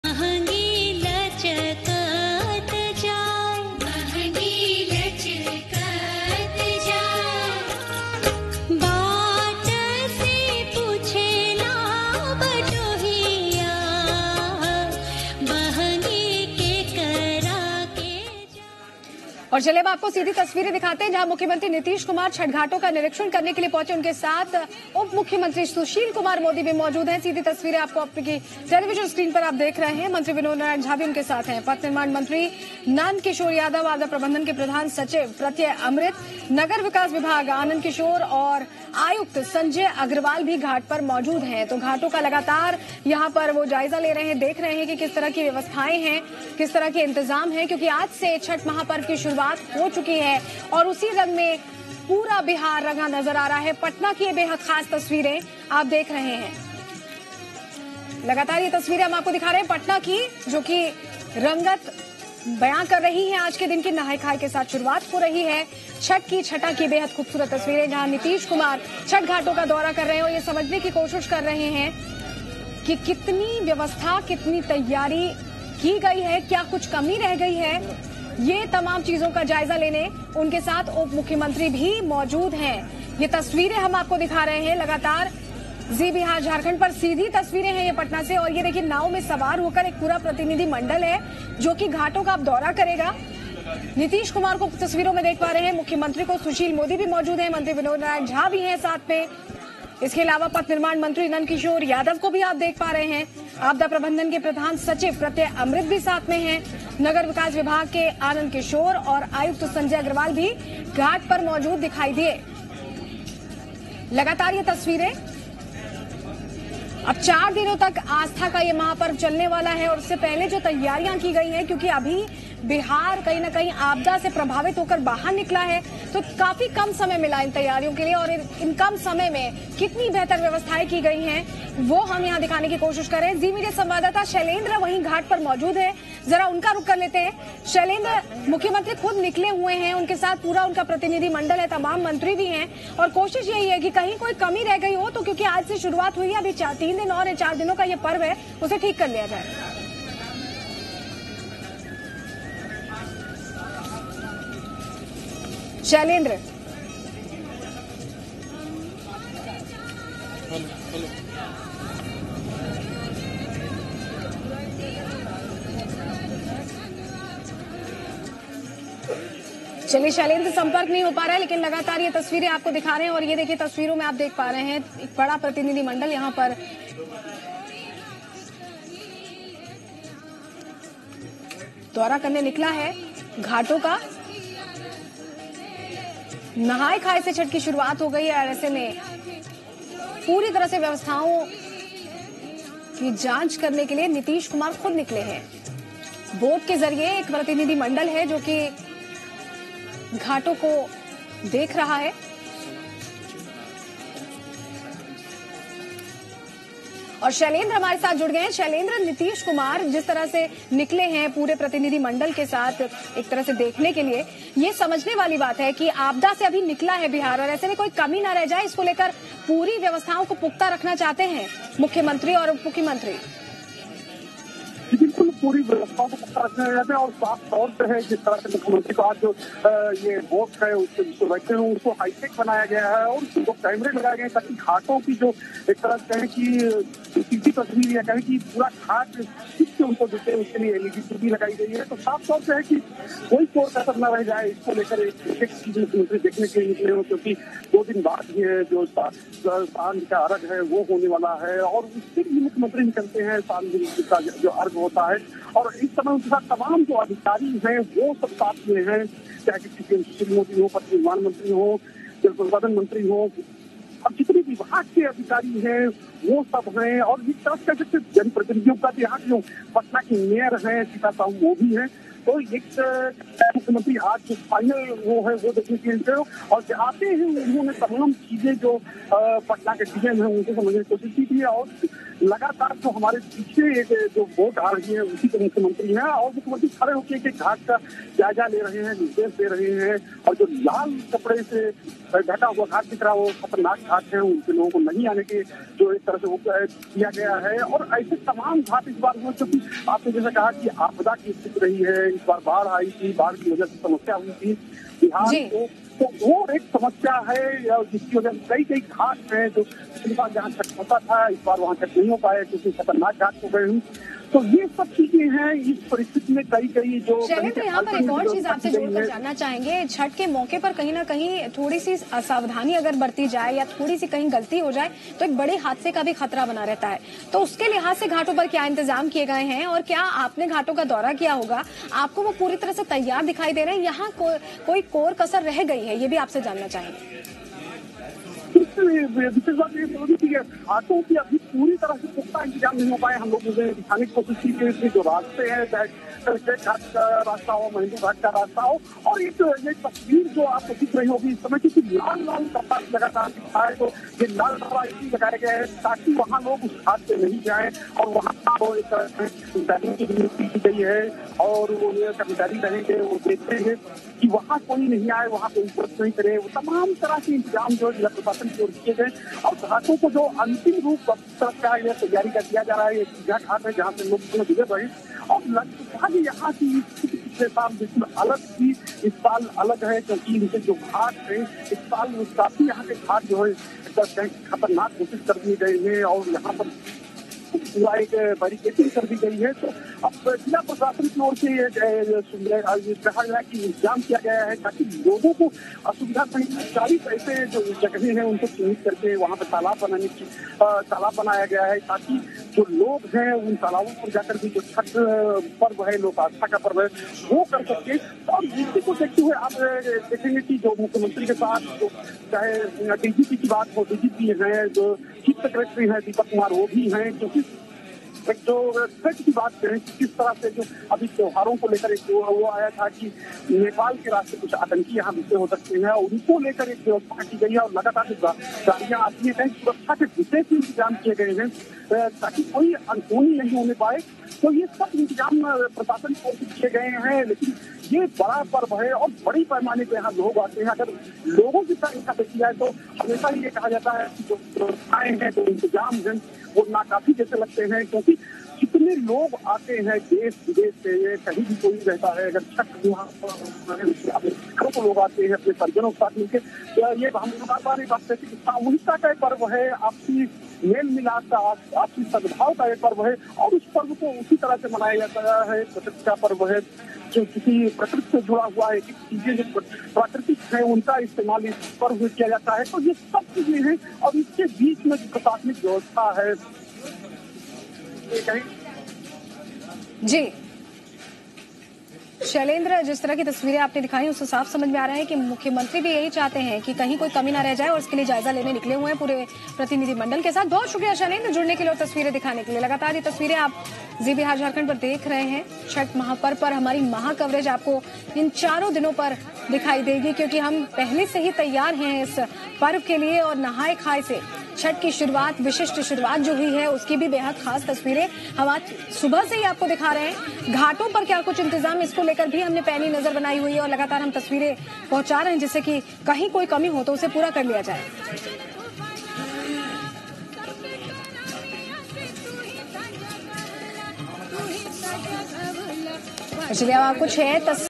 पटना: CM नीतीश ने किया छठ घाटों का निरीक्षण
छठ घाटों का निरीक्षण करने निकले मुख्यमंत्री नीतीश कुमार. डिप्टी सीएम सुशील मोदी, मंत्री नंद किशोर यादव समेत तमाम अधिकारी भी साथ.